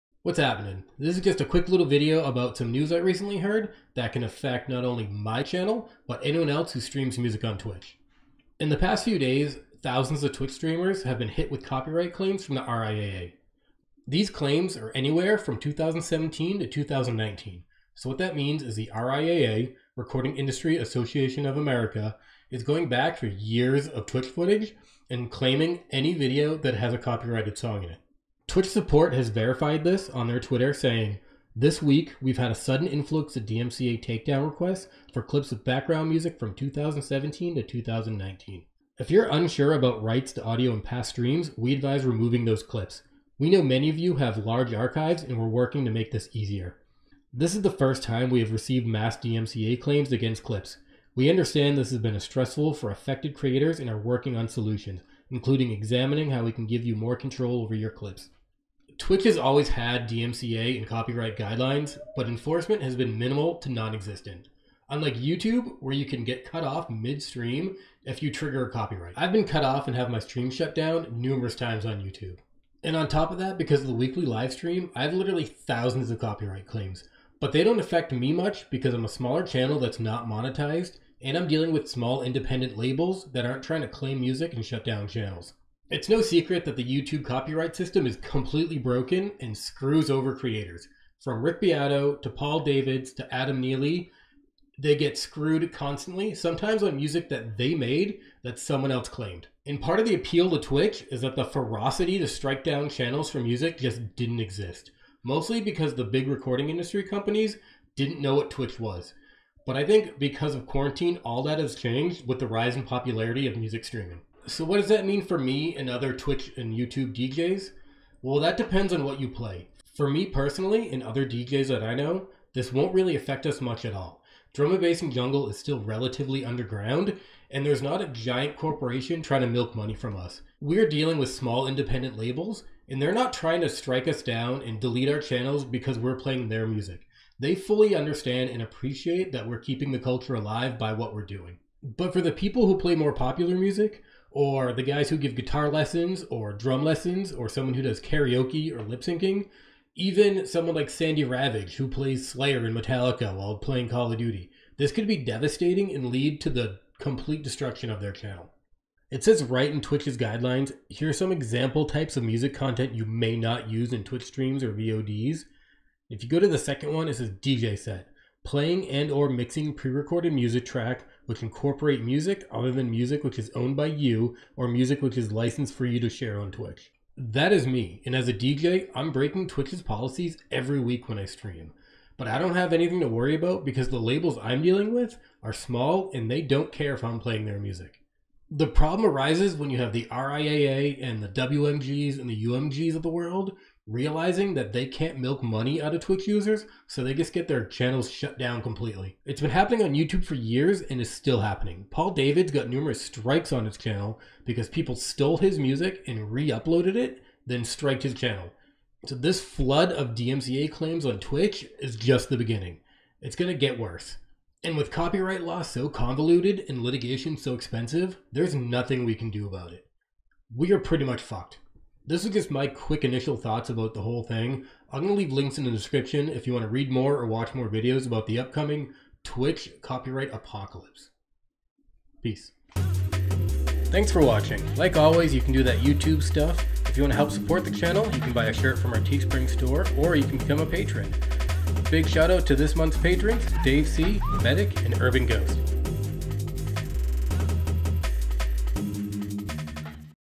Twitch DMCA Flood Could End DJ Channels
Recently Twitch has been flooded with dmca claims from the RIAA. This could be devastating for the dj community on the platform. These are my thoughts on the matter based on my perspective. If Twitch goes the way of youtube then dmca abuse will run rampant and any music content at all will be claimed and entire channels can be deleted.

I def stole his title for the thumbnail but forgot exactly what it was and ended up getting it wrong.

Big ups to or patrons:
DaveC
mEDIK
Urban Ghost